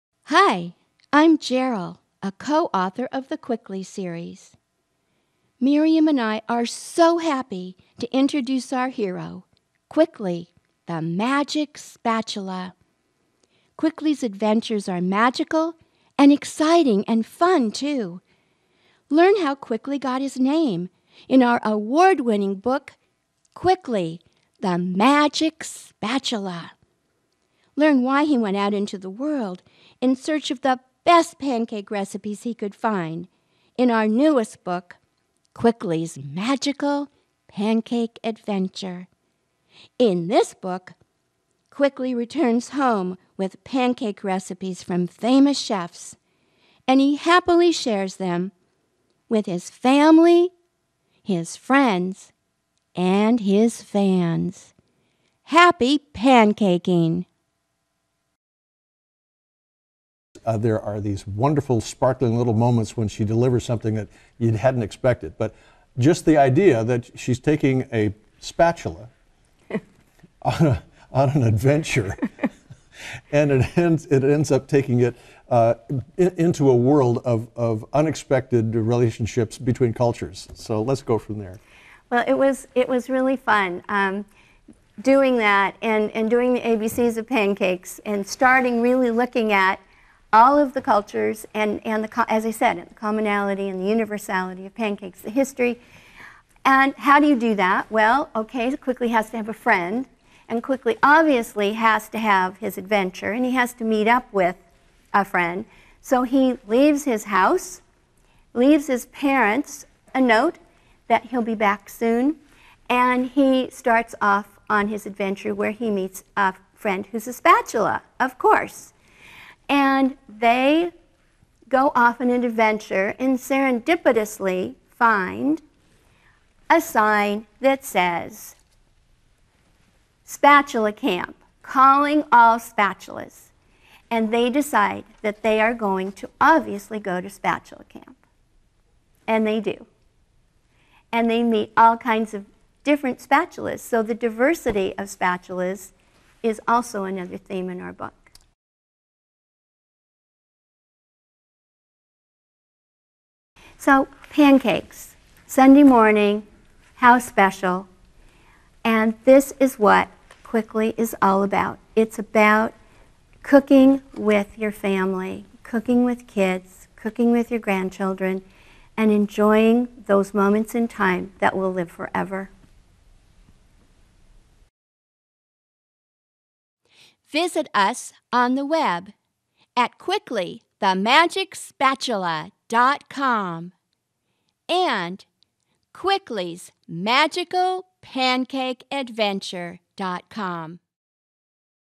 Quickly's Magical Pancake Adventure (The Quickly Series)
Award winning authors Miriam Kronish and Jeryl Abelmann once again bring their character Quickly: The Magic Spatula to life. Quickly's wish is to have a magical pancake adventure--to go out into the world in search of pancake recipes far and wide. This book blends Quickly's adventure with a treasure trove of delicious pancake recipes.